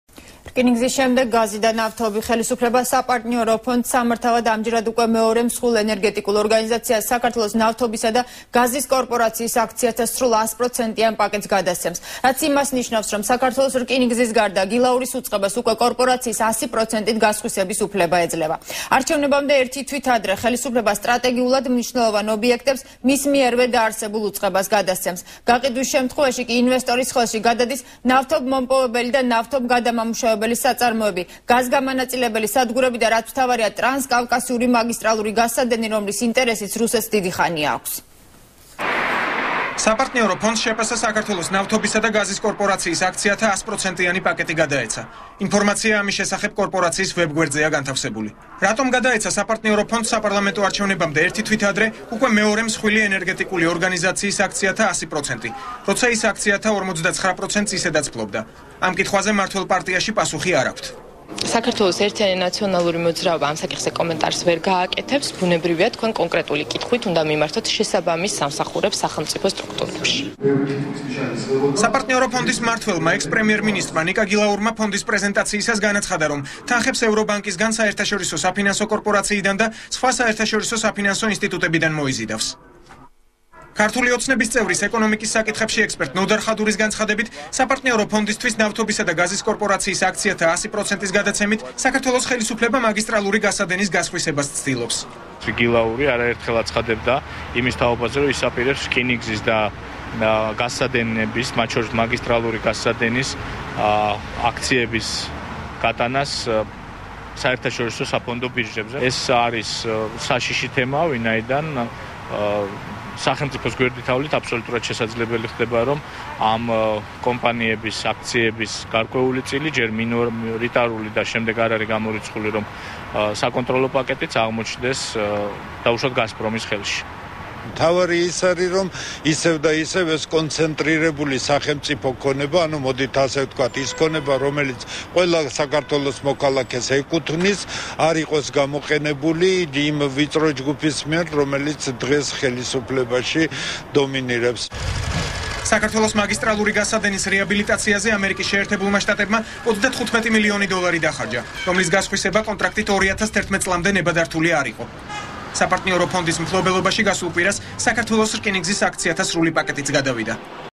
ახალი 9 | გილაურის ფონდის შენაძ. | 13.08
რკინიგზის შემდეგ გაზი და ნავთობი.
ხელისუფლება "საპარტნიორო ფონდს" სამართავად ამჯერად უკვე მეორე მსხვილ ენერგეტიკულ ორგანიზაციას -  "საქართველოს ნავთობისა და გაზის კორპორაციის" აქციათა სრულ, ას პროცენტიან პაკეტს გადასცემს. რაც იმას ნიშნავს რომ საქ-ოს რკინიგზის გარდა, გილაურის უწყებას უკვე  კორპორაციის ასი პროცენტით გასხვისების უფლება ეძლევა.
არჩევნებამდე ერთი თვით ადრე ხელისუფლება სტრატეგიულად მნიშვნელოვან ობიექტებს მის მიერვე დაარსებულ უწყებას გადასცემს.
გაყიდვის შემთხვევაში კი ინვესტორის ხელში გადადის ნავთობ მომპოვებელი და ნავთობ გადამამუშავებელი საწარმოები, გაზ გამანაწილებელი სადგურები და რაც მთავარია ტრანსკავკასიური მაგისტრალური გაზსადენი, რომლის ინეტერესიც რუსეთს დიდი ხანია აქვს.